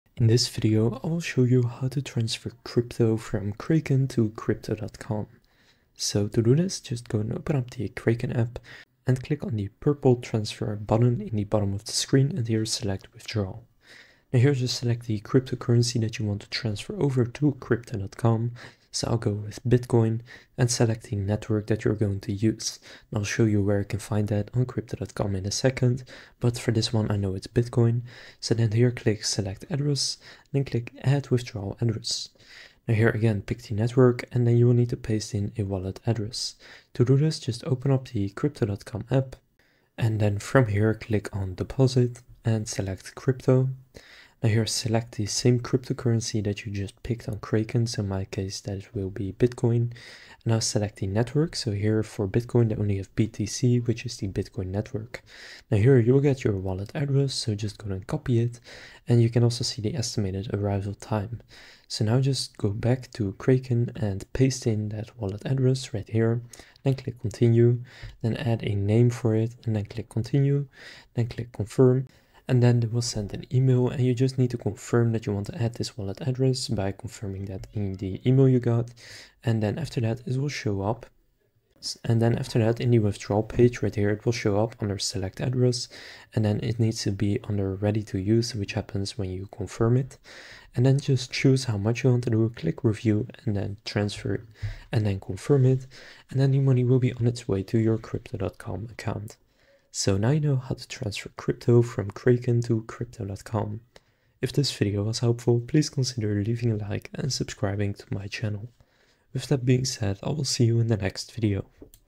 How to Transfer Crypto From Kraken to Crypto.com (2026)
In this video I'll show you how to Transfer Crypto From Kraken to Crypto.com.